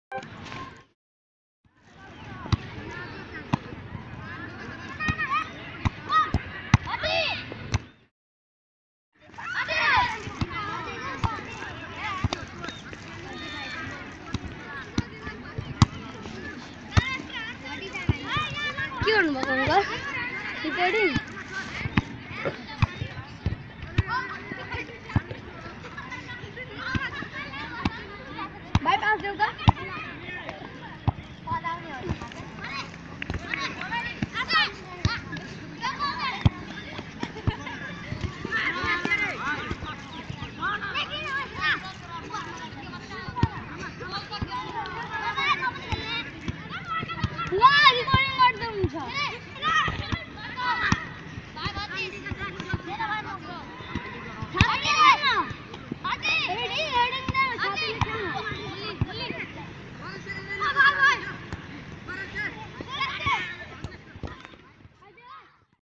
Amazing Football skills from 6 year old child in Duhabi Nepal
Football skills from 6 year old child in Duhabi Nepal